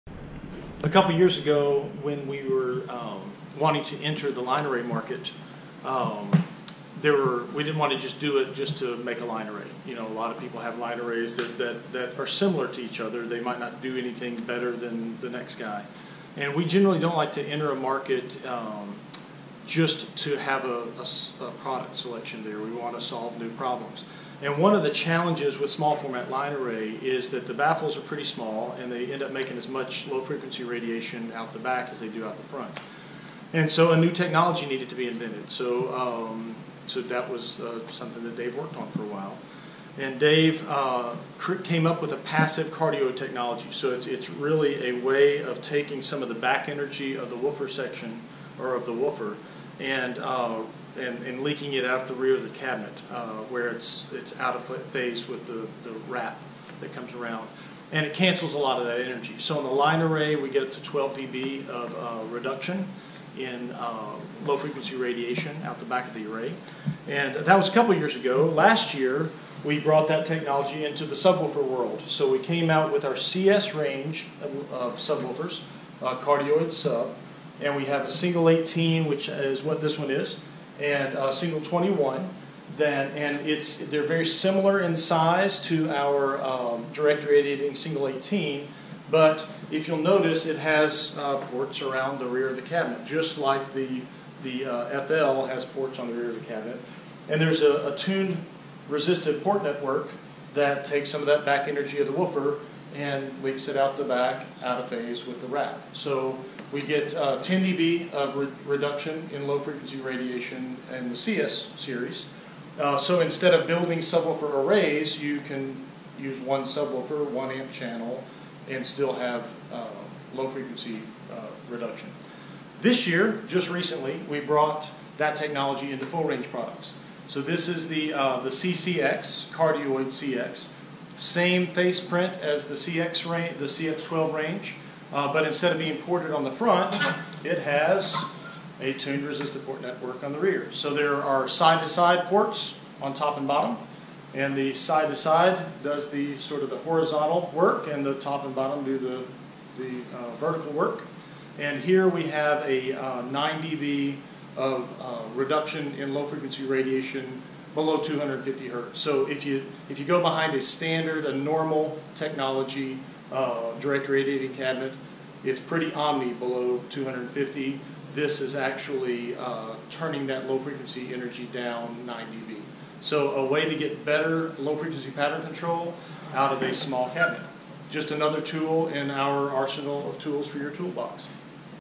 Fulcrum Acoustic Passive Cardioid Loudspeaker Models
Todd Foster describes our Passive Cardioid product evolution from line array modules to subwoofers to full-range loudspeakers. Since this video was produced in 2017, we've released even more passive cardioid models - see our full product offering

This video is a re-upload to @FulcrumAcoustic official. Original release date: 12/20/17.